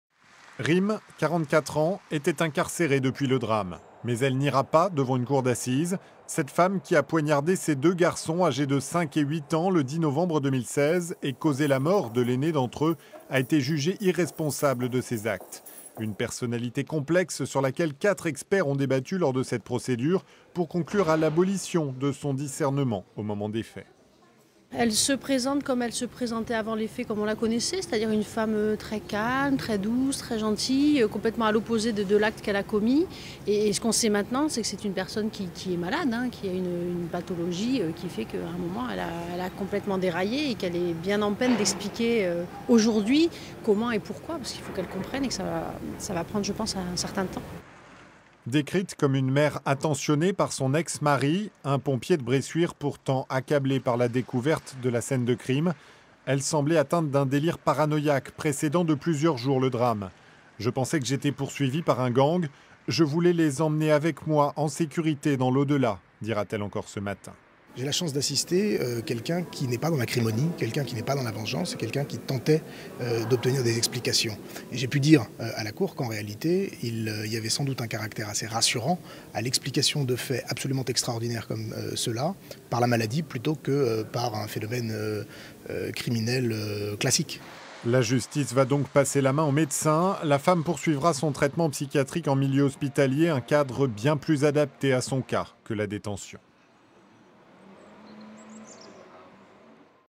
La mère de famille qui avait poignardé ses enfants à Bressuire est déclarée irresponsable
Tanguy Scoazec et José Sousa ont assisté à l'audience.

Intervenants : Me Patricia Coutand, Avocate de Rim B; Me Lee Takhedmit, Avocat du père et du fils survivant